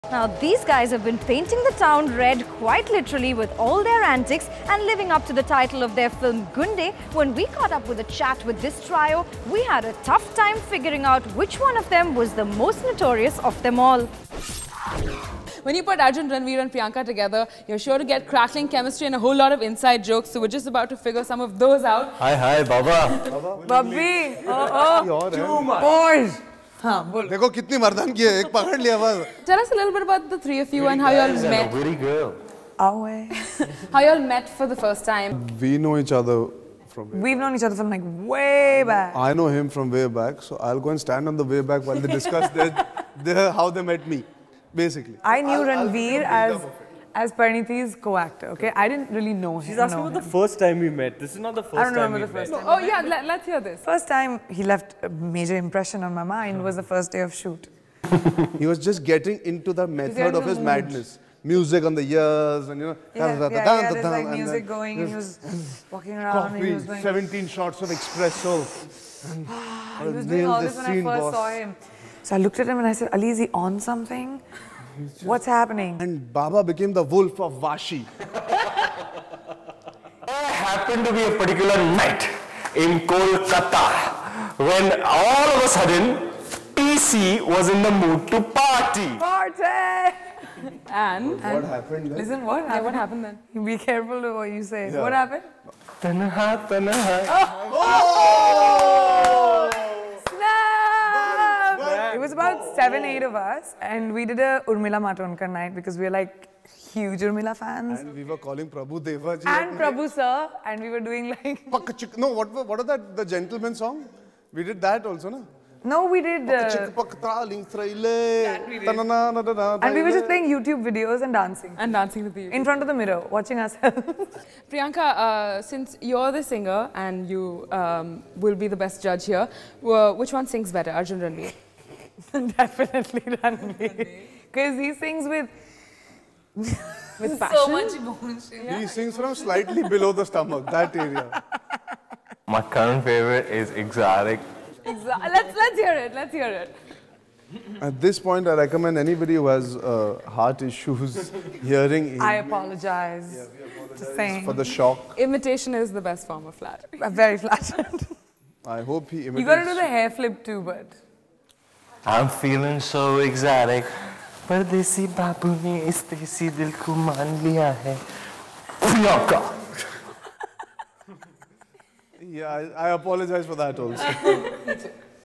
Meet the awesome Threesome!
E NOW team meets the cast of action crime thriller film 'Gunday'. Download the Times Now India's Election HQ app and get all the election info at one go. Social Media Links